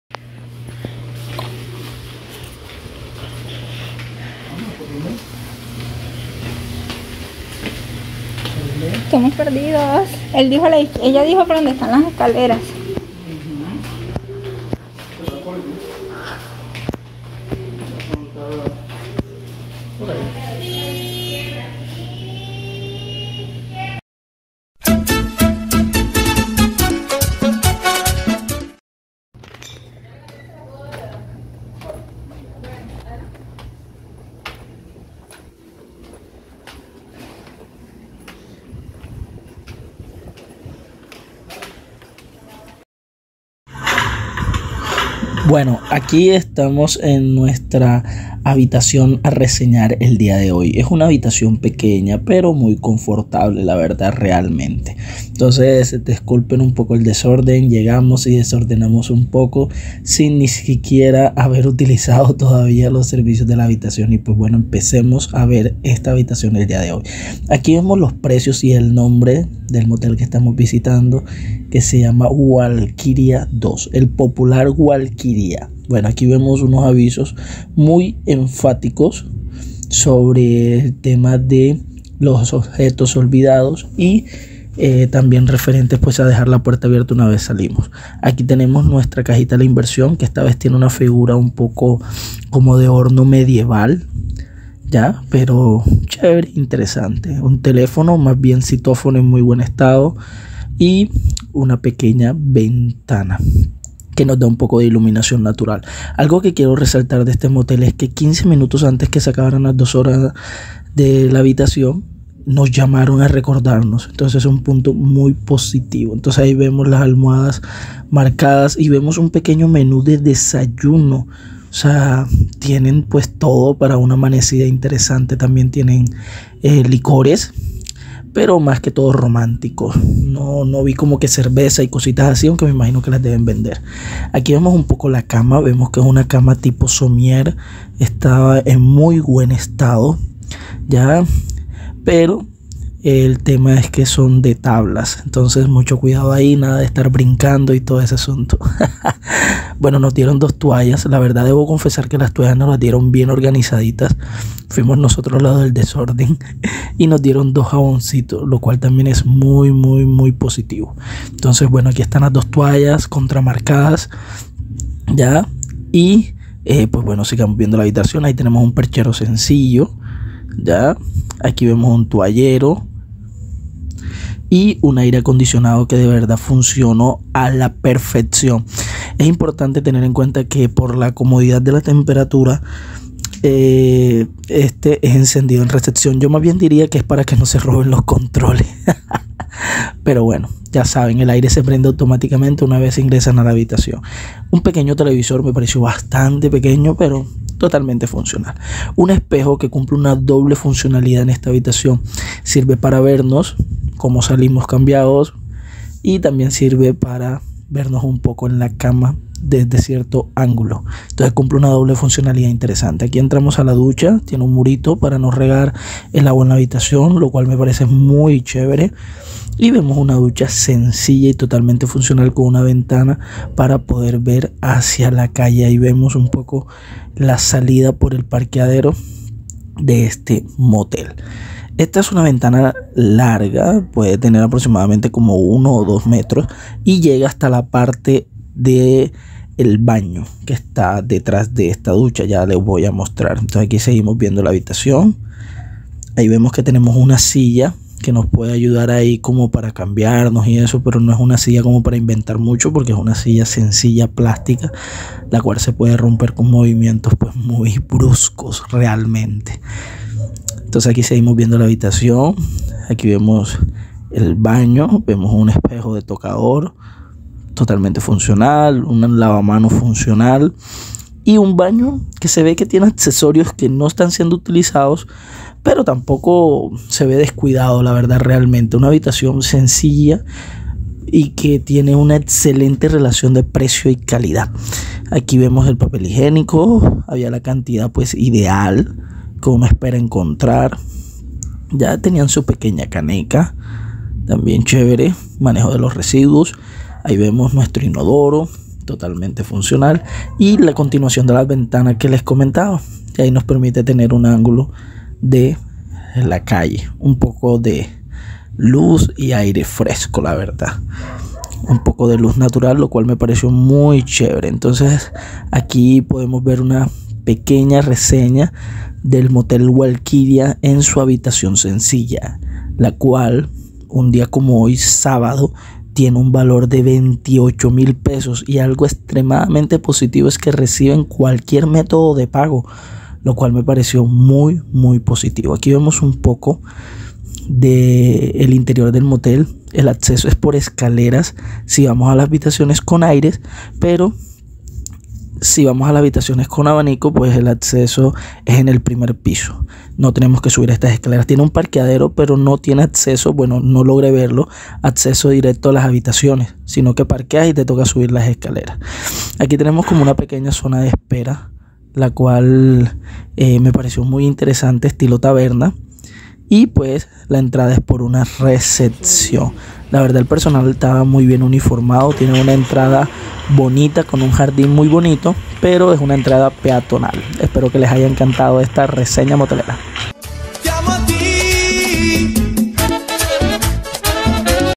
Reseña Motelera # 10 - Habitacion sencilla Walkiria 2
En esta ocasion reseñaremos un historico y popular motel que de seguro ha hecho parte de la vida universitaria de mas de un estudiante de las universidades y corporaciones cercanas al parque de los fundadores en la ciudad de Barranquilla, este motel es de esos que tienen fachada de yo no fui puesto a leguas parece un simple hogar pero solo hay qe detallarlo unos minutos para darse cuenta que el flujo inusual de personas entrando y saliendo denotan algo mas que una simple casa. En Mr M nos encontramos un establecimiento agradable, organizado y con uma combinacion entre historia y modernidad.